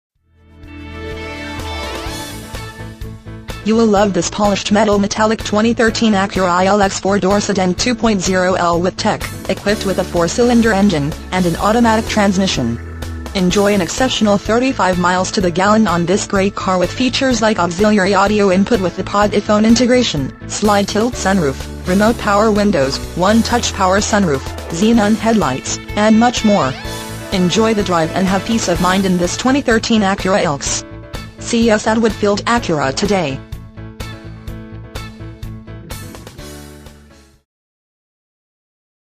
2013 Acura ILX Woodfield Acura Hoffman Estates, IL 60195
Acura ILX Check out this Polished Metal Metallic Acura Ilx 4 Dr Sedan, equipped with a 4 Cylinder engine and an automatic transmission. Enjoy an exceptional 35 miles to the gallon on this great car with features like Auxiliary Audio Input - Ipod/Iphone Integration, Slide/Tilt Sunroof, Remote Operation Power Windows, One-Touch Power Sunroof, Xenon Headlights, Push-Button Start Keyless Ignition, Homelink Universal Garage Door Opener, Auto-Dimming Inside Rearview Mirror, Variable Power-Assisted Steering, Remote Anti-Theft Alarm System, Heated Exterior Mirrors With Integrated Turn Signals, Electronic Brakeforce Distribution, Speed Sensitive Front Wipers, Front Fog Lights, Hard Drive Navigation System W/Voice Activation, Leather Upholstery, Power Glass Sunroof, Single Cd/Dvd Player In Dash, 8 Way Power Driver Seat, Heated Driver and Passenger Seating, and much more. Enjoy the drive and have peace of mind in this Acura Ilx. See us at Woodfield Acura today!
MILES: 15,979
VIN:19VDE1F75DE009571

Woodfield Acura
1099 w. Higgins Rd.